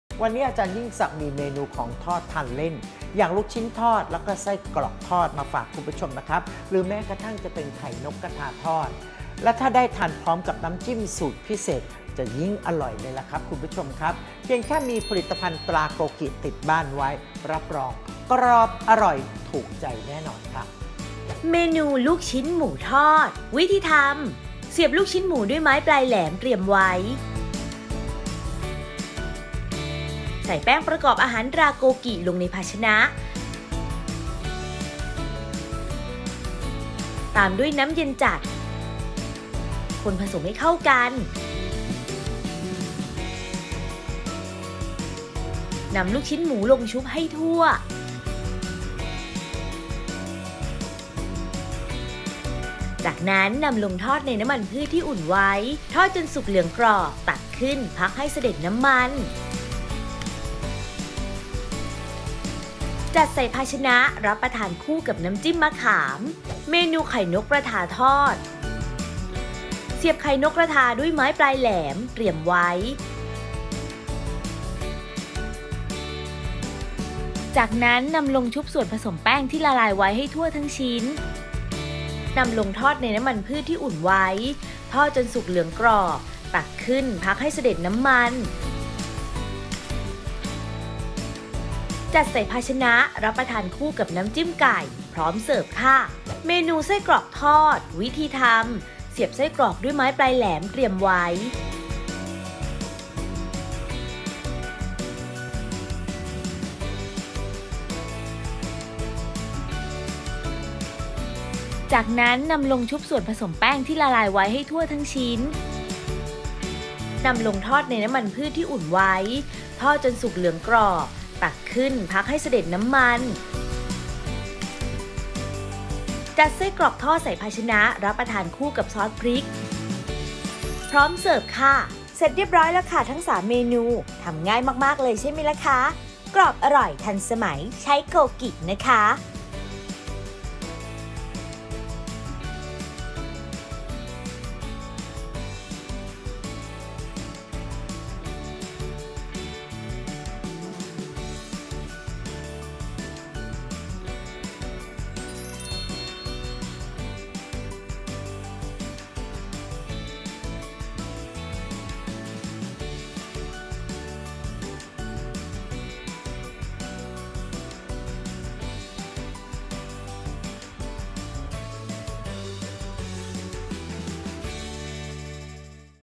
ลูกชิ้นหมูทอด / ไข่นกกระทาทอด / ไส้กรอกทอด ทำง่ายๆ สไตล์ อ. ยิ่งศักดิ์
ลูกชิ้นหมูทอด ผลิตภัณฑ์:แป้งประกอบอาหาร ตราโกกิ

ลูกชิ้นหมู  200 กรัม
แป้งประกอบอาหาร ตราโกกิ 150 กรัม
น้ำเย็นจัด  210 กรัม
น้ำจิ้มมะขาม สำหรับรับประทานคู่

1.เสียบลูกชิ้นหมูด้วยไม้ปลายแหลม เตรียมไว้
2. ใส่แป้งประกอบอาหาร ตราโกกิลงในภาชนะ ตามด้วยน้ำเย็นจัดคนผสมให้เข้ากัน นำลูกชิ้นหมูลงชุบให้ทั่ว
3.จากนั้นนำลงทอดในน้ำมันพืชที่อุ่นไว้ ทอดจนสุกเหลืองกรอบ ตักขึ้น พักให้สะเด็ดน้ำมัน
4.จัดใส่ภาชนะ รับประทานคู่กับน้ำจิ้มมะขาม

ไข่นกกระทาทอด ผลิตภัณฑ์:แป้งประกอบอาหาร ตราโกกิ
ไข่นกกระทา  200 กรัม
น้ำจิ้มไก่ สำหรับรับประทานคู่

1.เสียบไข่นกกระทาด้วยไม้ปลายแหลม เตรียมไว้
2.ใส่แป้งประกอบอาหาร ตราโกกิลงในภาชนะ ตามด้วยน้ำเย็นจัด คนผสมให้เข้ากัน นำไข่นกกระทาลงชุบให้ทั่ว
3.จากนั้นนำลงทอดในน้ำมันพืชที่อุ่นไว้ ทอดจนสุกเหลืองกรอบ ตักขึ้น พักให้สะเด็ดน้ำมัน
4.จัดใส่ภาชนะ รับประทานคู่กับน้ำจิ้มไก่ พร้อมเสิร์ฟ

ไส้กรอกทอด ผลิตภัณฑ์:แป้งประกอบอาหาร ตราโกกิ
ไส้กรอก   200 กรัม
ซอสพริก สำหรับรับประทานคู่

1.เสียบไส้กรอกด้วยไม้ปลายแหลม เตรียมไว้
2.ใส่แป้งประกอบอาหาร ตราโกกิลงในภาชนะ ตามด้วยน้ำเย็นจัด คนผสมให้เข้ากัน นำไส้กรอกลงชุบให้ทั่ว
3.จากนั้นนำลงทอดในน้ำมันพืชที่อุ่นไว้ ทอดจนสุกเหลืองกรอบ ตักขึ้น พักให้สะเด็ดน้ำมัน
4.จัดไส้กรอกทอดใส่ภาชนะ รับประทานคู่กับซอสพริก พร้อมเสิร์ฟ

▲Line @yingsakfood